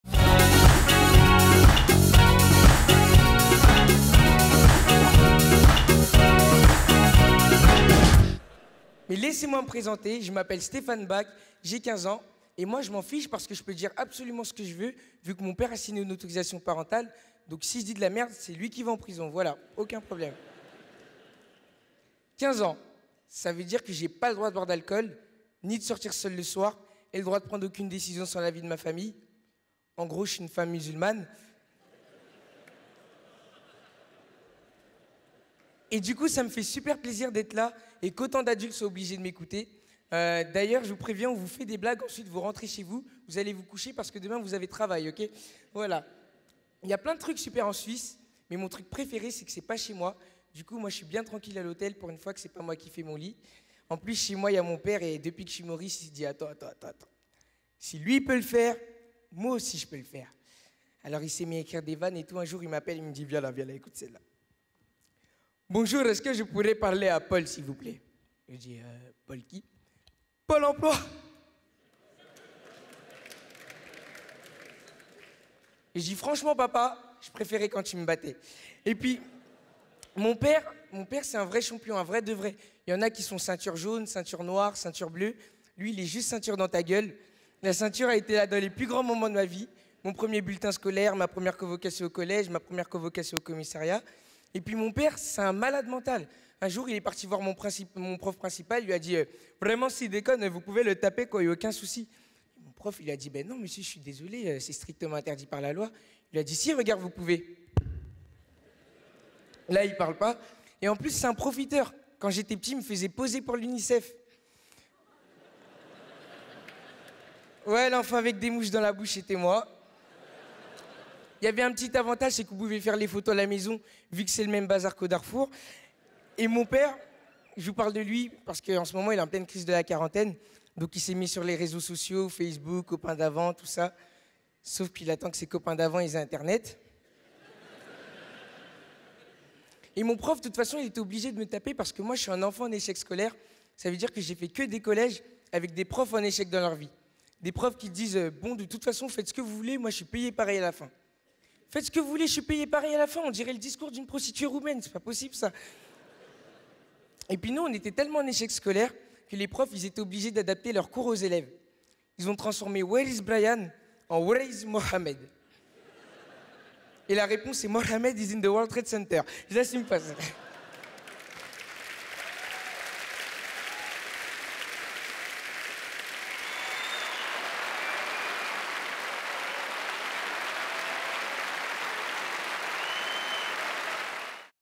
Stéphane Bak - Histoire de famille...
Le très jeune et talentueux Stéphane Bak monte sur la scène de Montreux pour parler des rapports particuliers qu'il entretien avec son père... Humour corrosif en perspective !